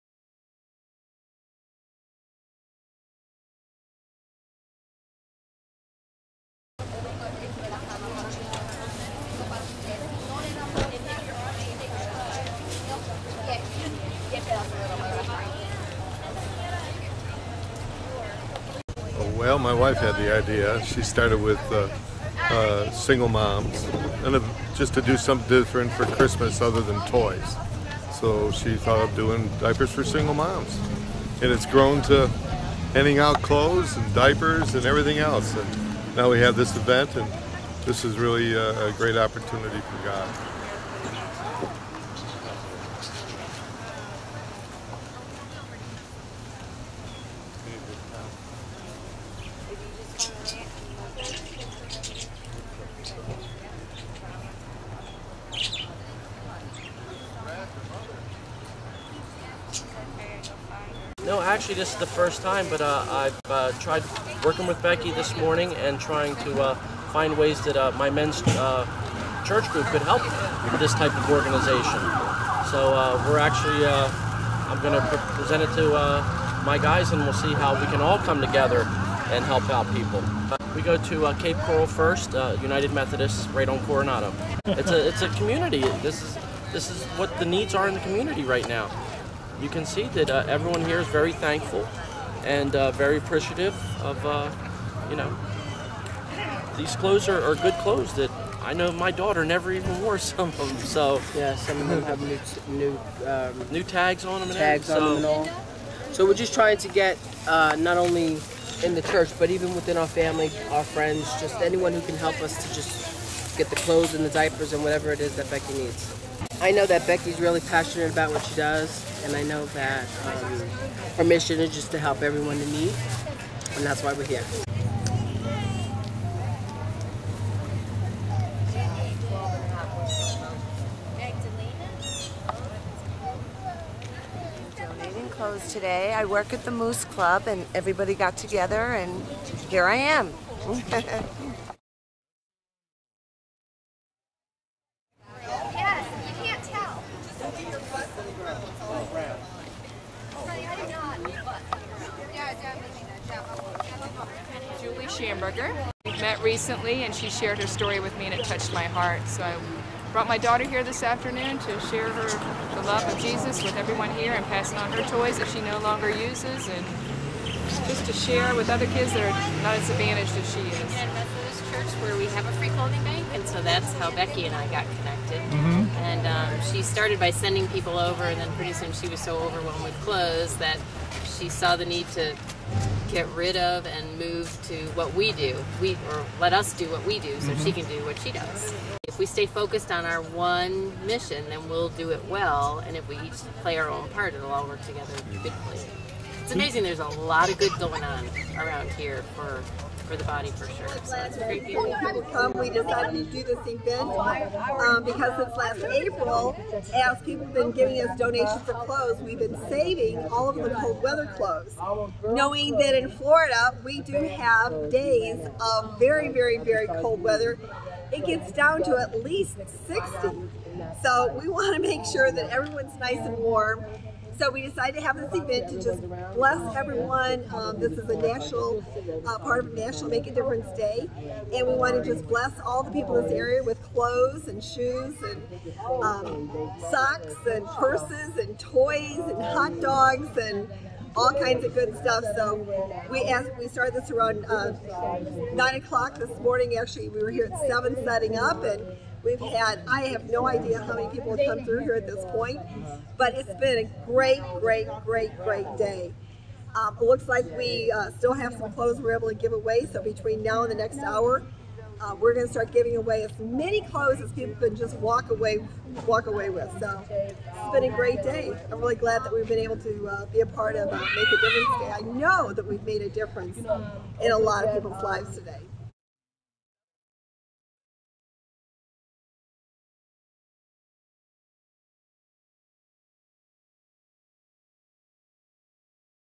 National Make A Difference Day - Destiny Diaper Bank
Jack and Becky Hines are special people who help the most marginalized of folks.  On "National Make A Difference Day," October 24, 2009, they gave away free clothing, shoes, toys and food!